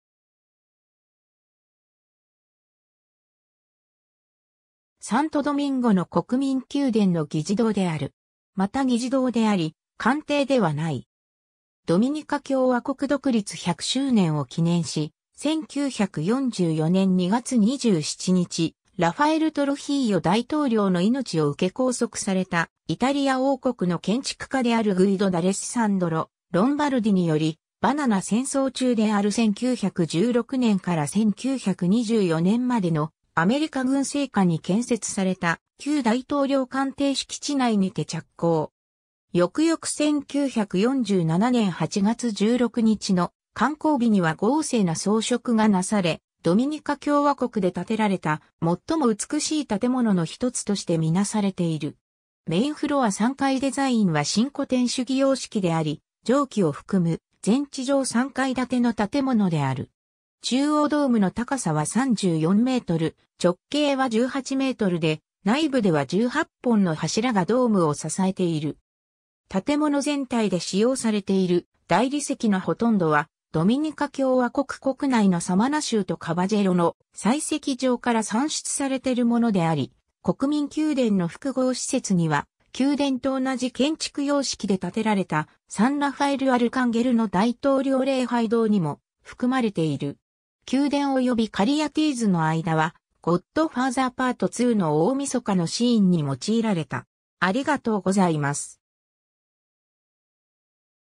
国民宮殿 (ドミニカ共和国)
サントドミンゴの国民宮殿（スペイン語: PalacioNacional , Palacio Nacional de la República Dominicana )は、1944年にイタリアの建築家によって建てられた、ドミニカ共和国のその国家行政府（議長及び副議長）の議事堂である。
 また議事堂であり、官邸ではない。
 ドミニカ共和国独立100周年を記念し、1944年2月27日ラファエル・トルヒーヨ大統領(当時)の命を受け拘束されたイタリア王国(当時)の建築家であるグイド・ダレッサンドロ・ロンバルディにより、バナナ戦争中である1916年から1924年までのアメリカ軍政下に建設された旧大統領官邸(スペイン語:MansiónPresidencial)敷地内にて着工。
 翌々1947年8月16日の完工日には豪勢な装飾が為され、ドミニカ共和国で建てられた最も美しい建物の1つとして見なされている。
 メインフロア 3階 デザインは新古典主義様式であり、上記を含む全地上3階建ての建物である。
 中央ドームの高さは34m、直径は18mで、内部では18本の柱がドームを支えている。
建物全体で使用されている大理石の殆んどはドミニカ共和国国内のサマナ州とカバジェロの採石場から産出されてるものであり、国民宮殿の複合施設には宮殿と同じ建築様式で建てられたサンラファエルアルカンゲルの大統領礼拝堂にも含まれている。
 宮殿及びカリヤティーズの間は、ゴッドファーザー PART IIの大晦日のシーンに用いられた。